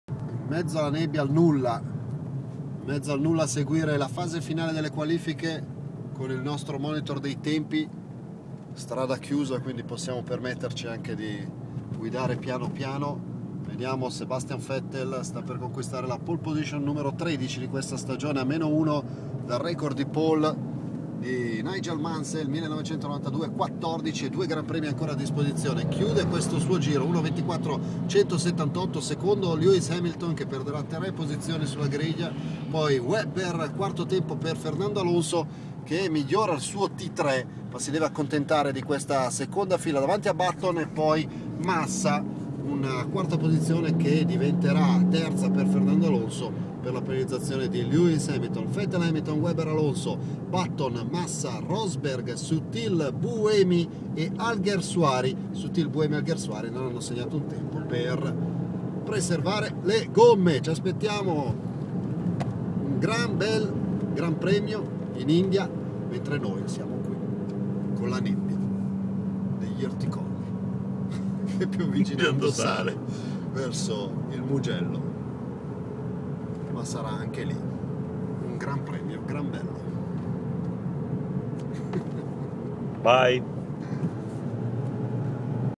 Qualifica india
Pole di vettel in mezzo alla nebbia del mugello